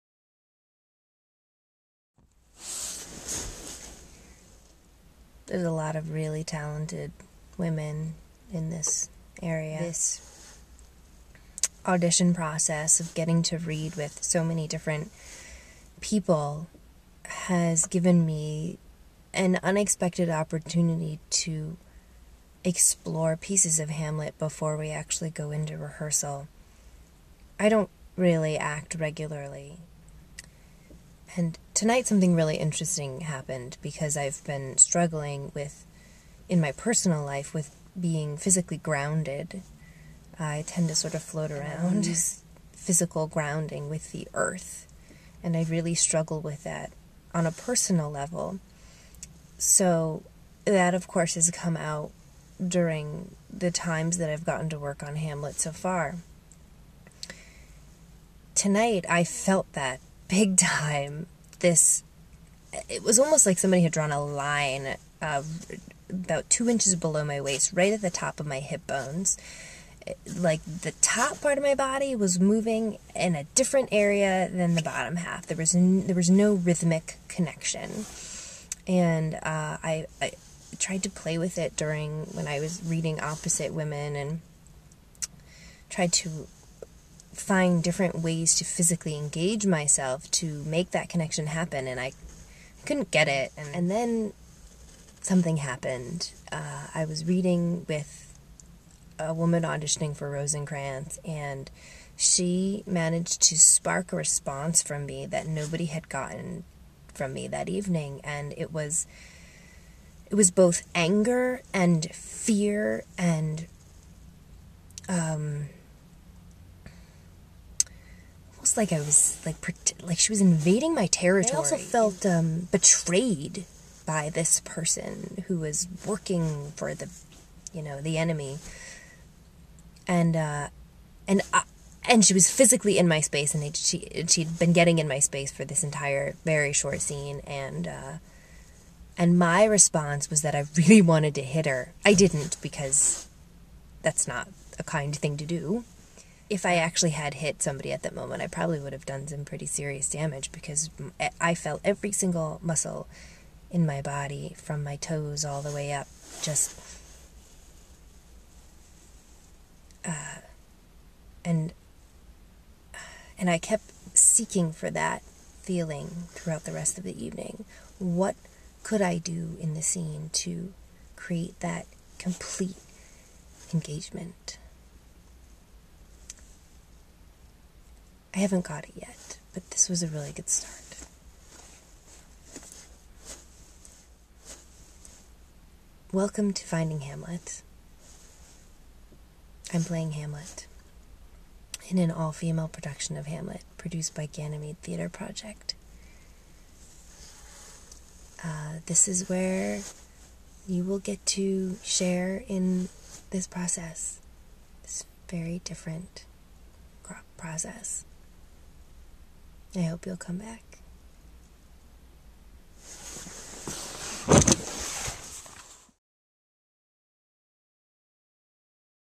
Finding Hamlet - 1
FindingHamlet kicks off their blog with an entry from the title player after the final round of auditions.  "Hamlet," featuring an all-female cast, opens in Lowell, MA on May 7 and then moves to Somerville, MA.